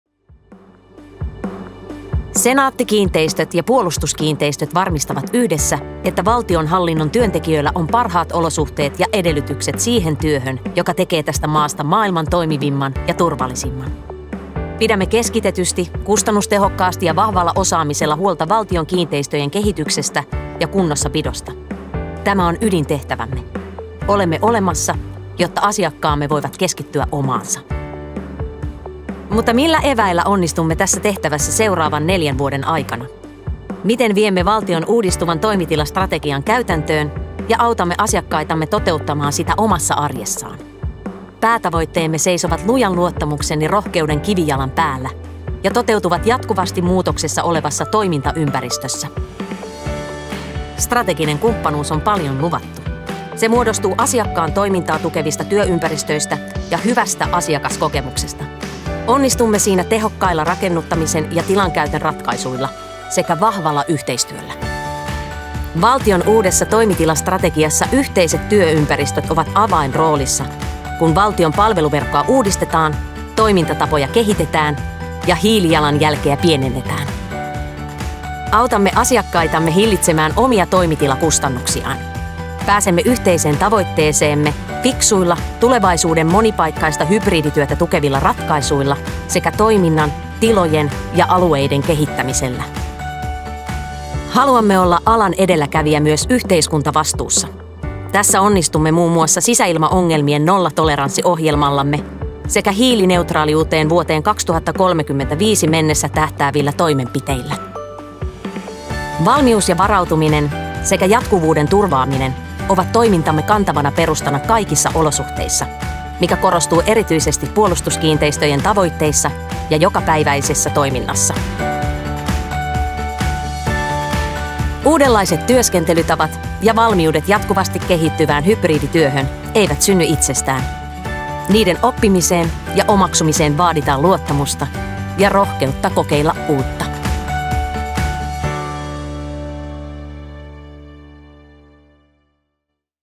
Senaatti-kiinteistöjen strategia 2022-2025
Senaatti-kiinteistöt ja Puolustuskiinteistöt varmistavat yhdessä, että valtionhallinnon työntekijöillä on parhaat olosuhteet ja edellytykset siihen työhön, joka tekee tästä maasta maailman toimivimman ja turvallisimman.